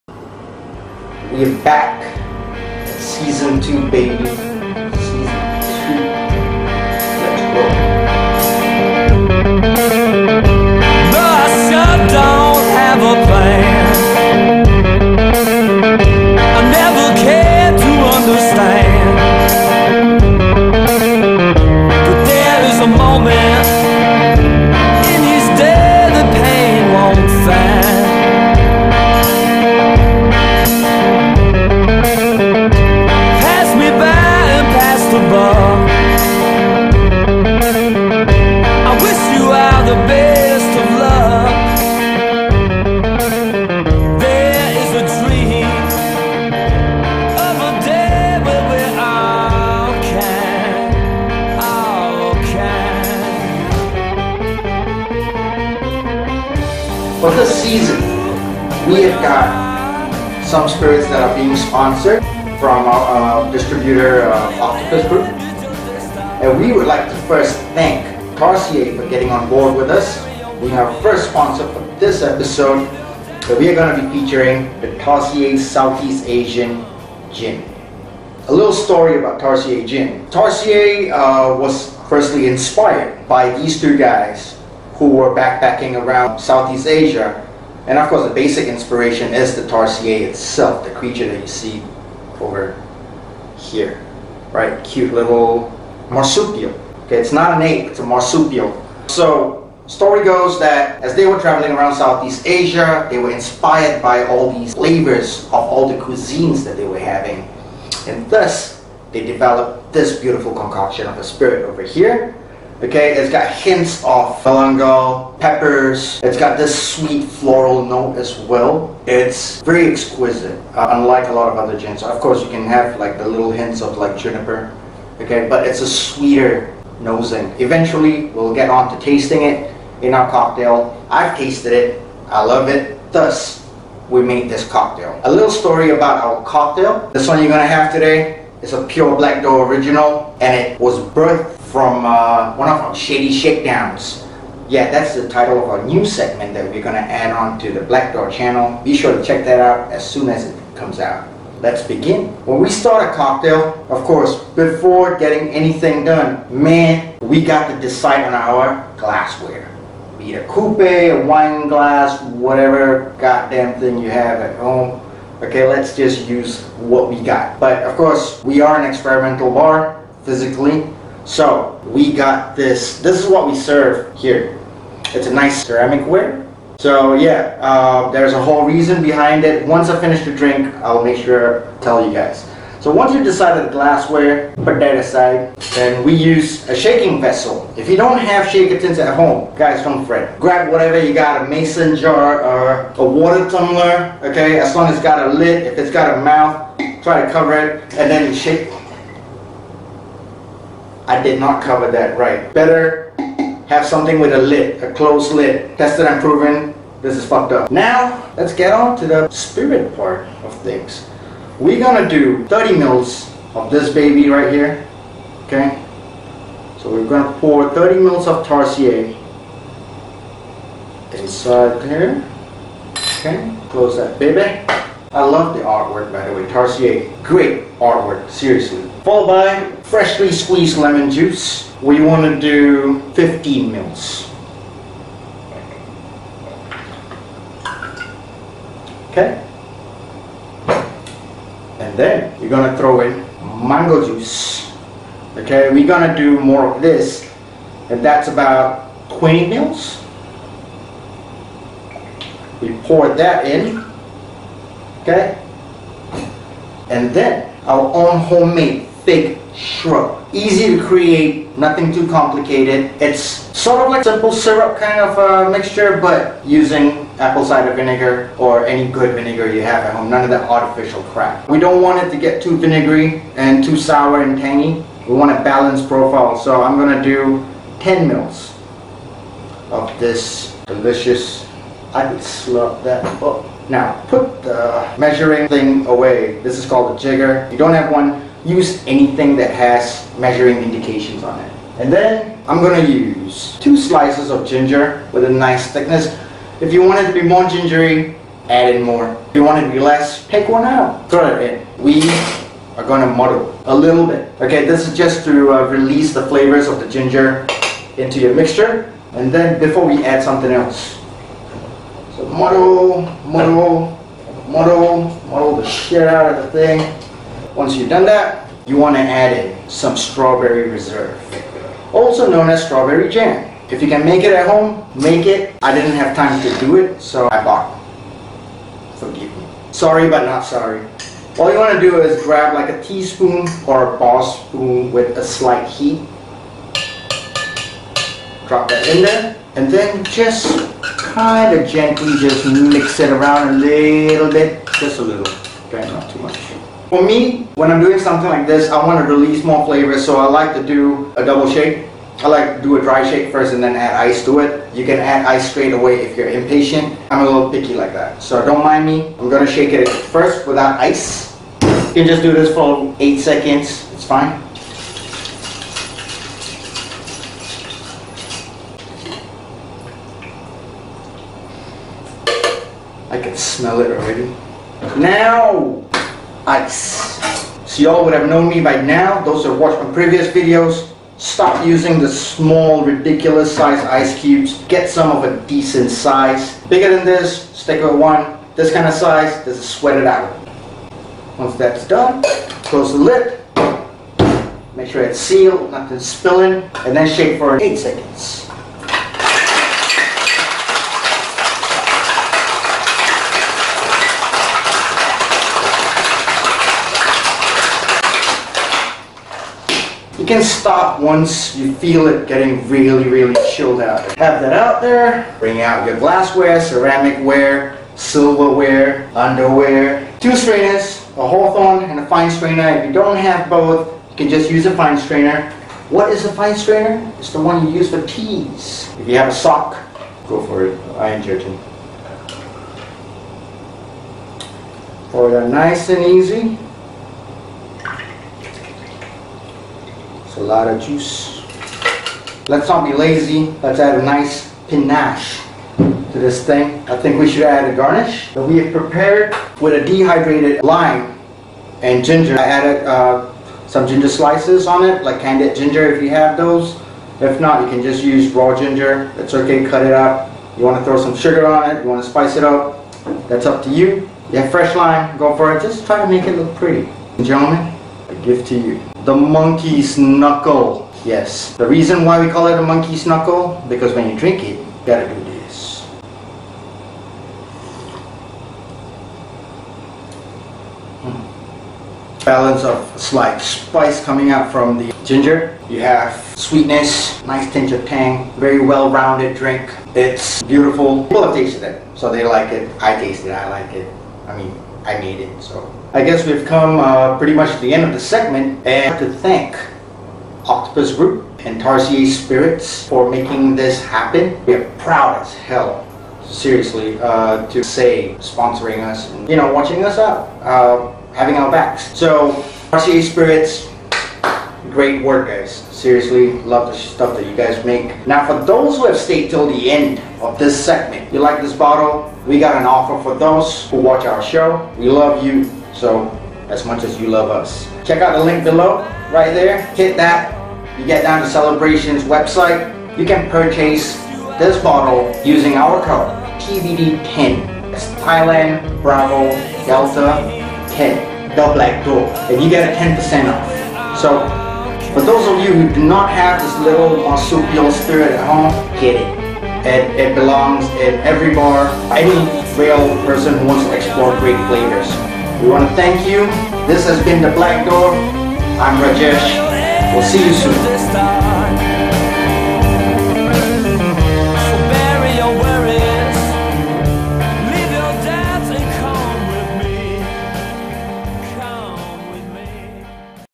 TBD Libations S02E01: Original Gin Cocktail, The Monkey's Knuckle
We took awhile as we wanted to bring you, our fans and guests the best we have to offer.
This cocktail we created is a true Black Door original. Birthed while filming for our second segment called, "Shady Shakedowns" which should be released pretty soon on our channel. We would love to introduce you to , "The Monkey's Knuckle", with beautiful delicious flavour notes ringing your senses.